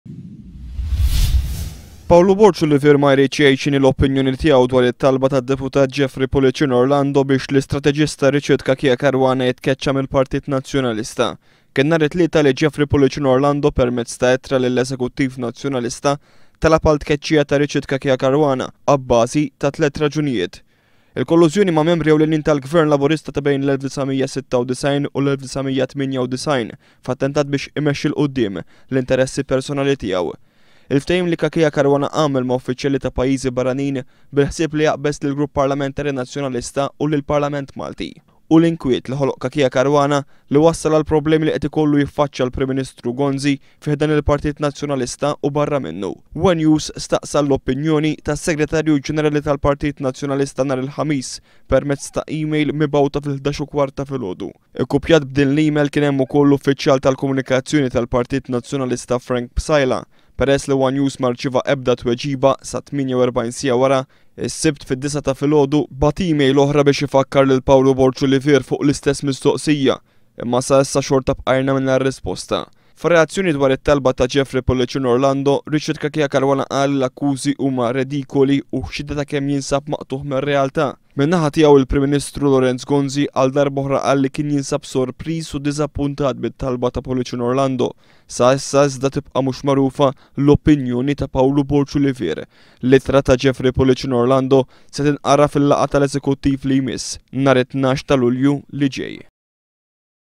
Pawlu Borg Olivier ma jridx jgħid x'inhi l-opinjoni tiegħu dwar it-talba ta' JPO.
Is-segretarju generali tal-Partit Nazzjonalista Pawlu Borg Olivier qed jinjora mistoqsijiet ta' ONE News dwar it-talba tad-deputat Jeffrey Pullicino Orlando biex Richard Cachia Caruana jitkecca mill-Partit Nazzjonalista.Pawlu Borg Olivier ma kellu ebda twegiba minkejja li ghal darba, darbtejn dan l-istazzjon talab ghal reazzjoni minghandu